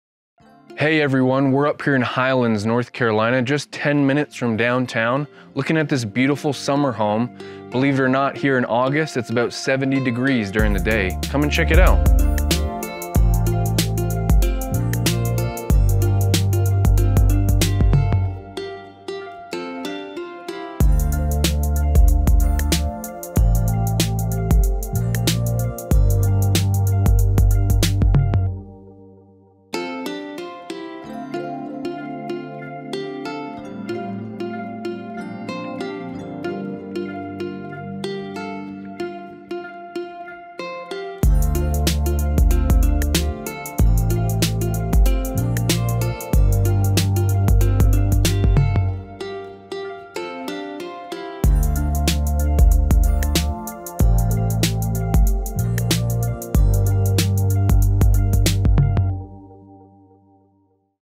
Highlands NC Cottage
🌞 Dreaming of your own summer escape? Look no further! 🏡 This charming cottage in Highlands, NC is now up for grabs – a rare opportunity you don't want to miss, just minutes away from downtown Main Street. 🛍️🍷

🏞️ Nestled in the picturesque Highlands, this cozy cottage offers the perfect blend of mountain tranquility and urban convenience. Imagine sipping your morning coffee on the porch, surrounded by nature's beauty, and yet being a stone's throw away from boutique shops and eateries. ☕🛍️

🌳 With its rustic elegance and modern comforts, this cottage is your gateway to a serene retreat. Whether you're seeking outdoor adventures or cozy evenings by the fireplace, this could be your slice of paradise. 🚴‍♂️🏕️

Don't wait! Seize this rare opportunity to own your own piece of Highlands magic. 🏡✨